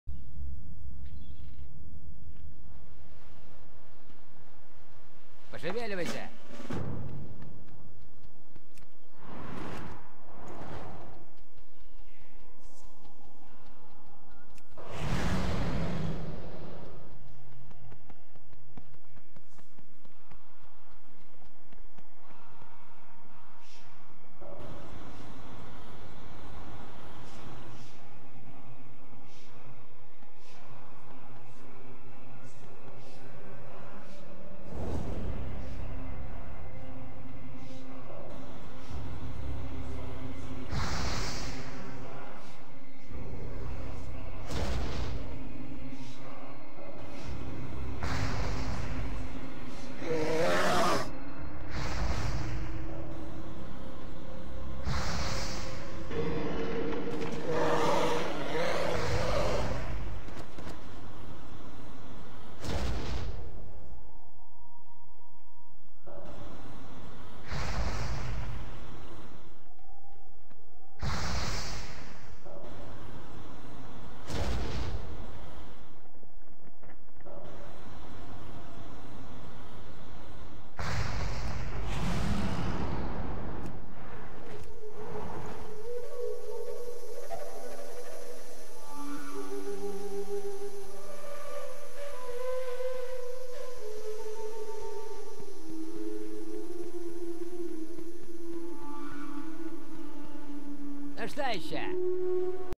Ледяной холод(Ice Cold)(id=14261)
Воспользуйтесь Криохладом 16, чтобы убить 8 агатовых драконов.